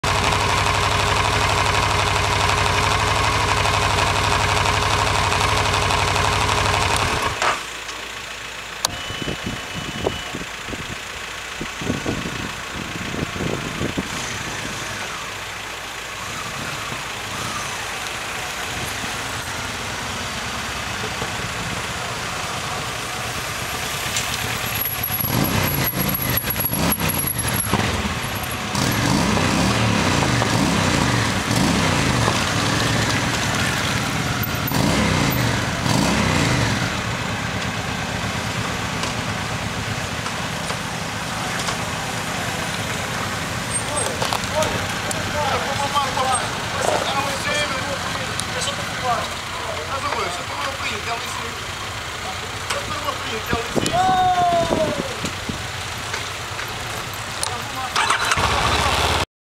2º Passeio MGM Paul da Serra/Fanal/R. da Janela (5)
Chegada dos Moto Guitas á Ribeira da Janela.